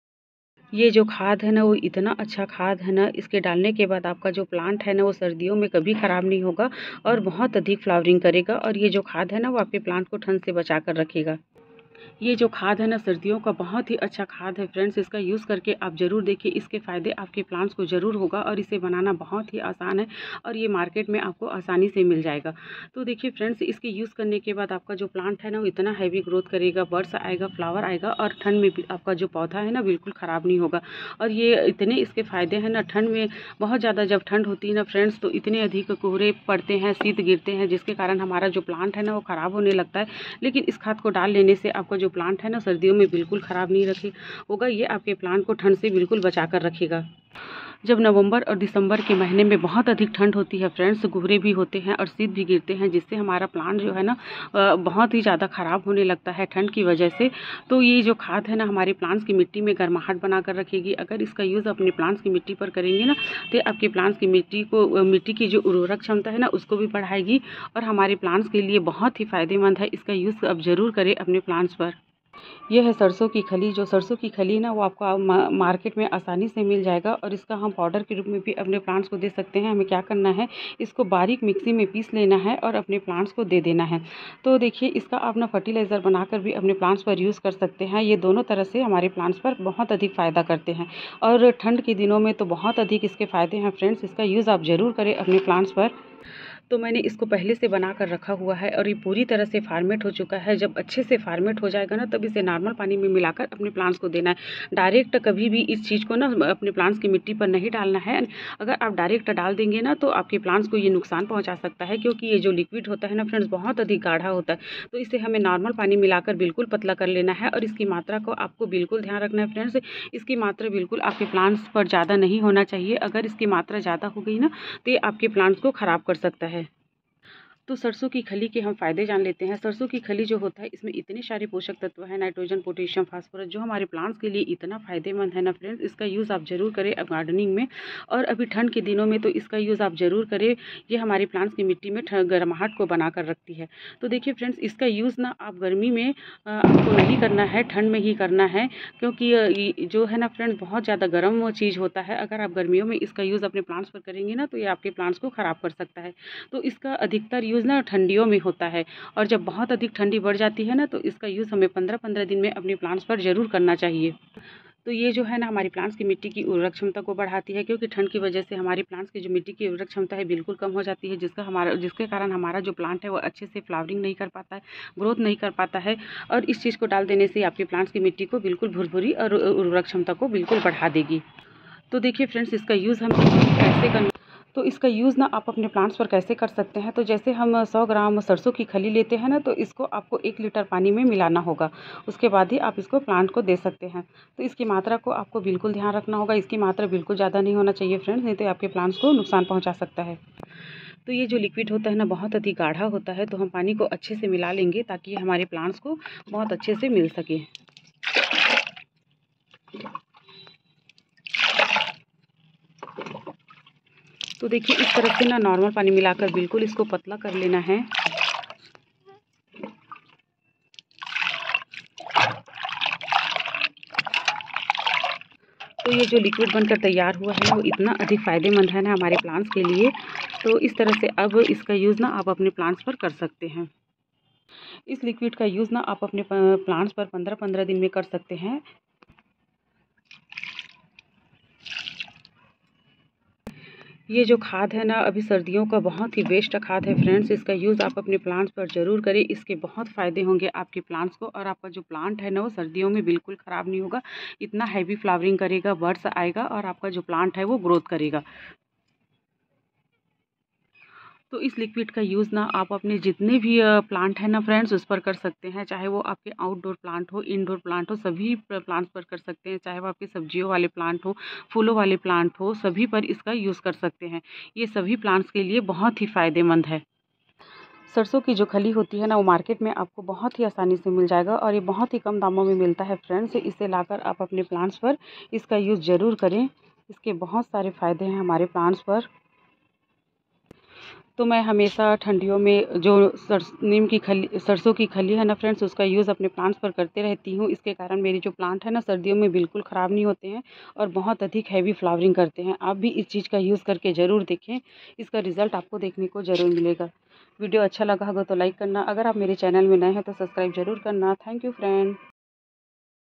सर्दियों में मरते सुखते पौधे को नयी ग्रोथ कैसे दें? | 100% असरदार तरीका
15 दिनों पर एक कप डालें ठंड में पौधे ना सूखेगें ना मरेगें | तेजी से बढ़ेगें. @COMFORT_GROWING_5M